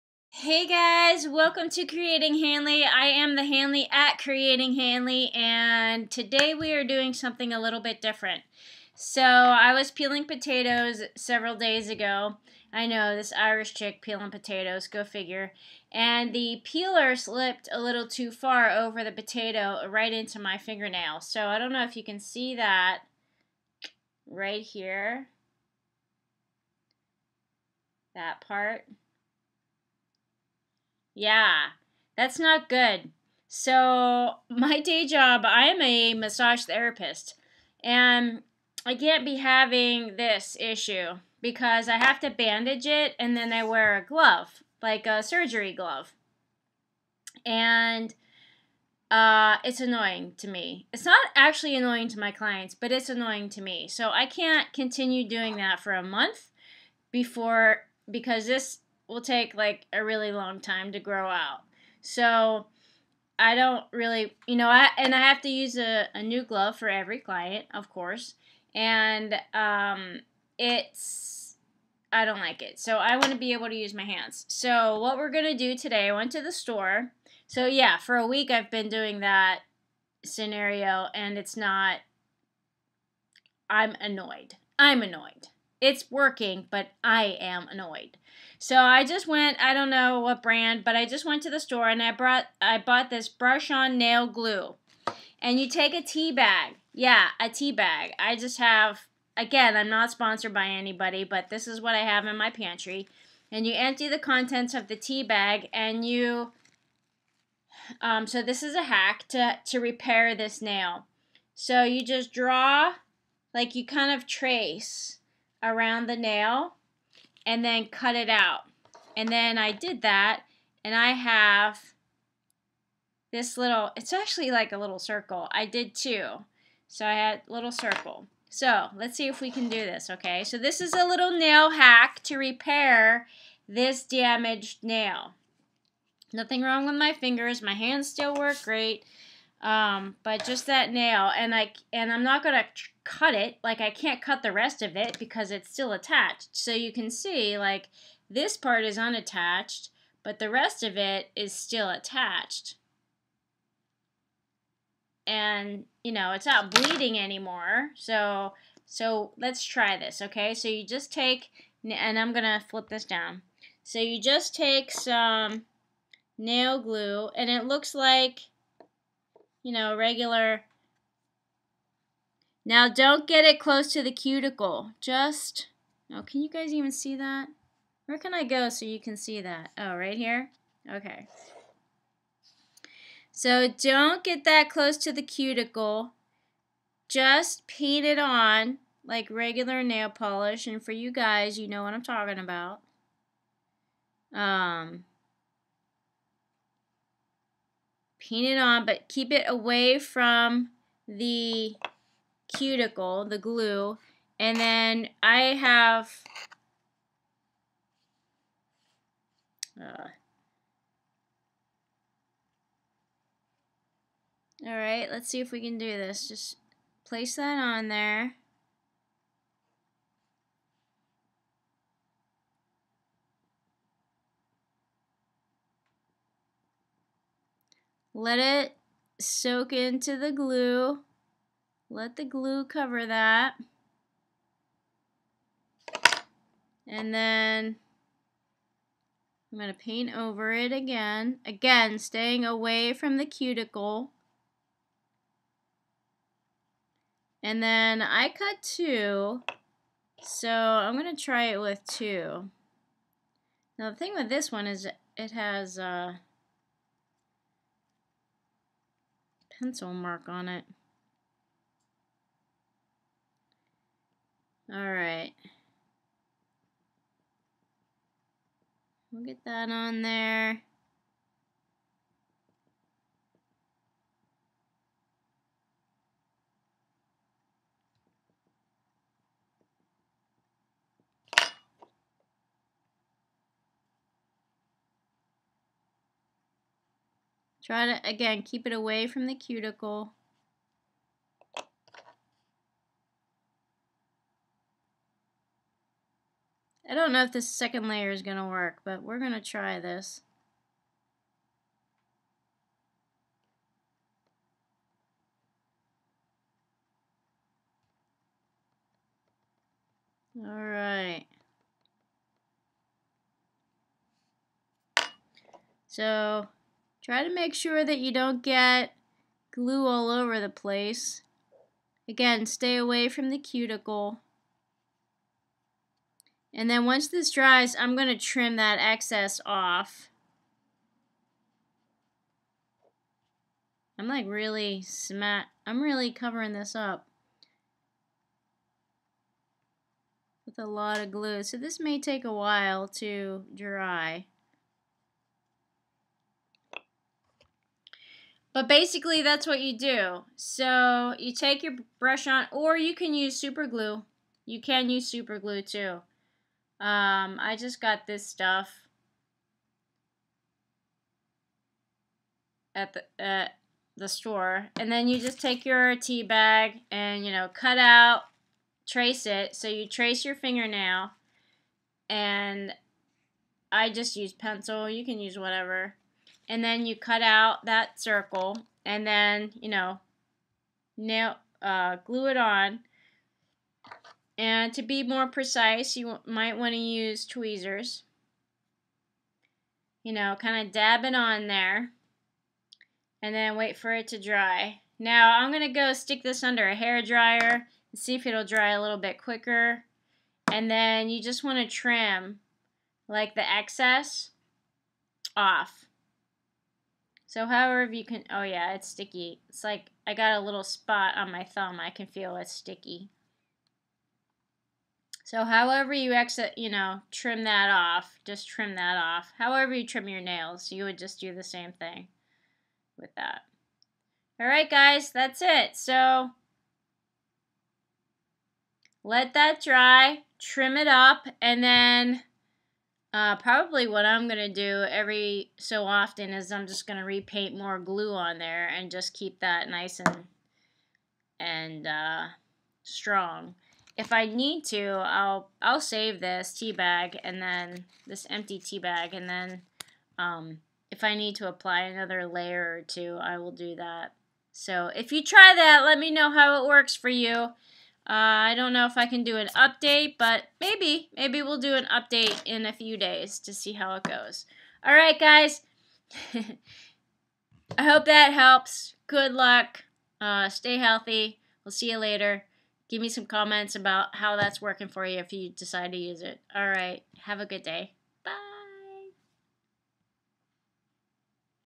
Repair Fingernail Hack
@CreatingHanley ripped a fingernail peeling potatoes, and now it needs repaired. You can use superglue or brush-on nail glue and a tea bag to fashion a quick new nail that will hold strong while your original nail takes proper time to grow out and renew itself. See how it's done in this video.